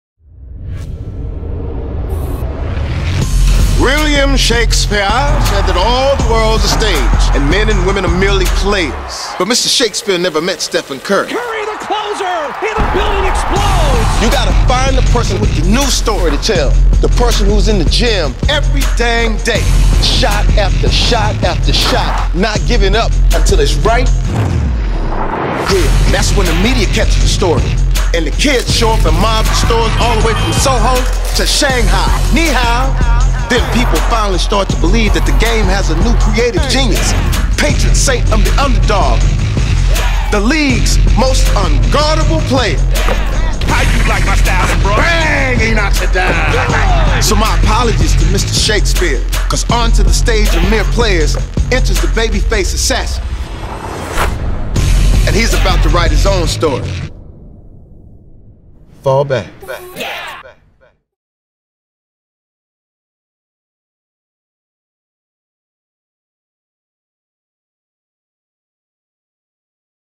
Charged By Belief - Lo últimos de Under Armour y Stephen Curry
El crack de los Warriors vuelve a convertirse en referente de Under Armour para su nuevo spot.